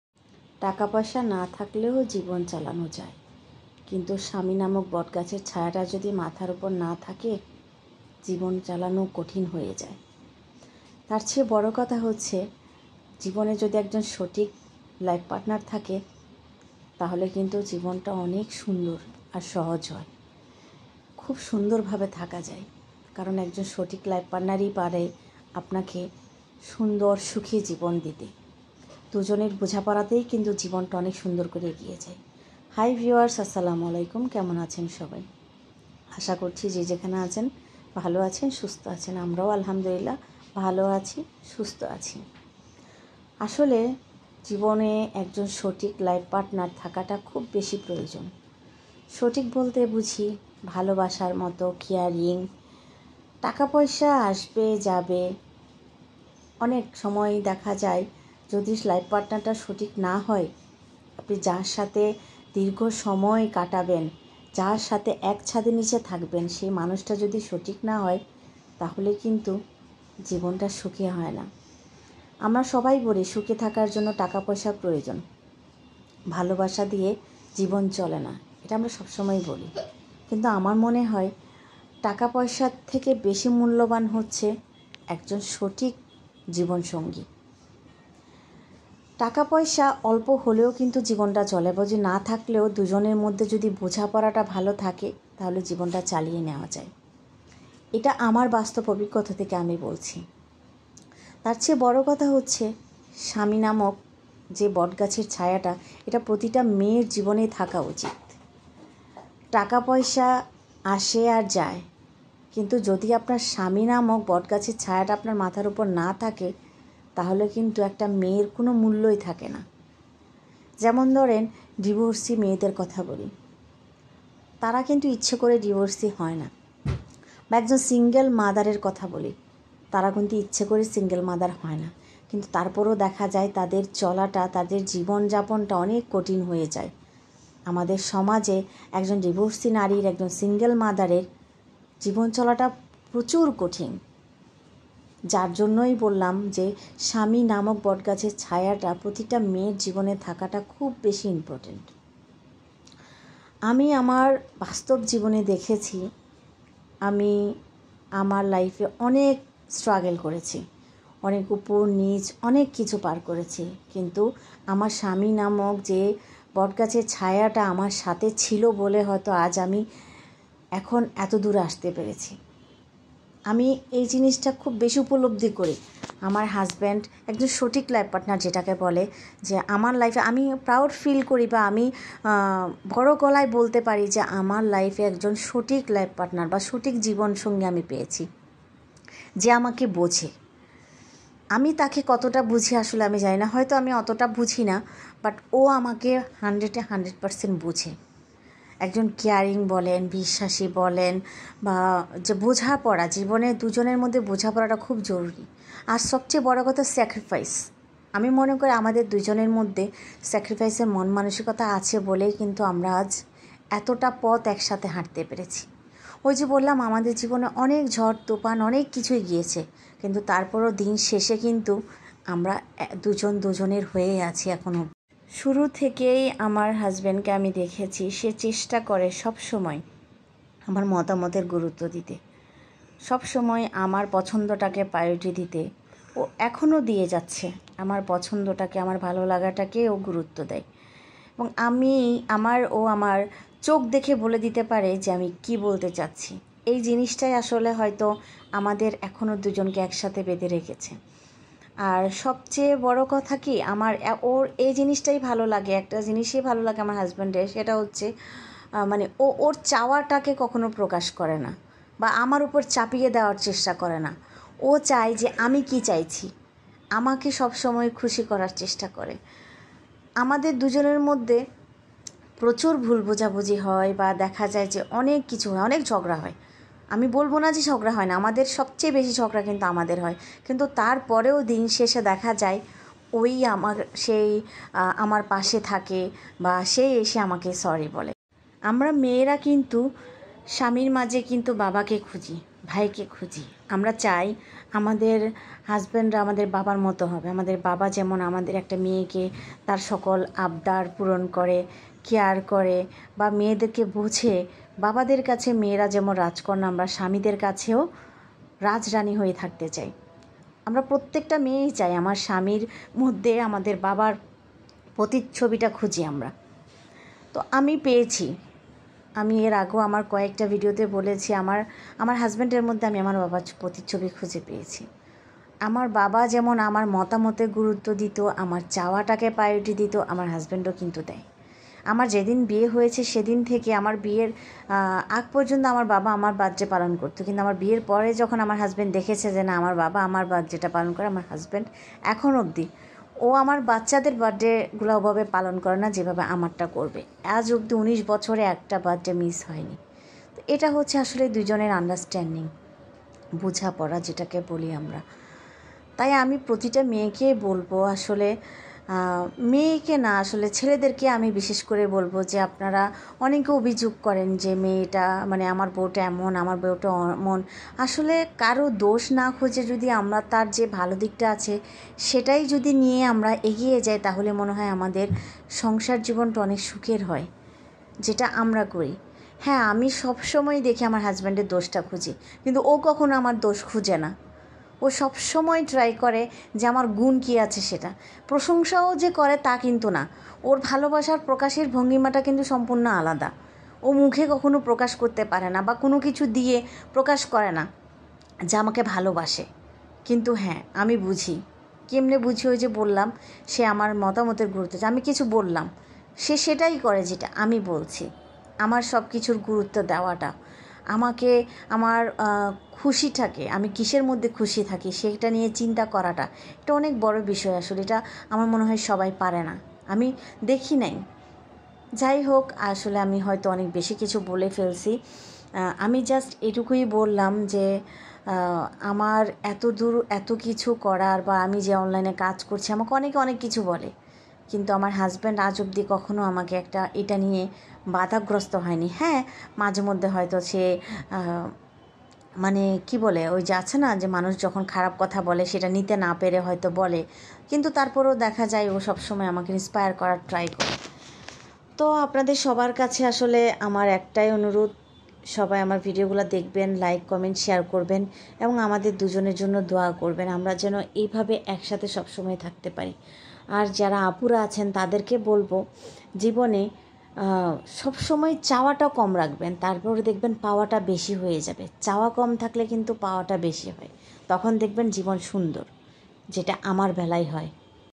একজন নারীর জন্য বিয়ের পর সব চেয়ে মূল্যবান হলো তার স্বামী||একজন সঠিক জীবনসঙ্গী থাকা মানে আপনি সুখী
Plz like my page👉RK Moon vlog

Follow my instrgram 👉Rehana karim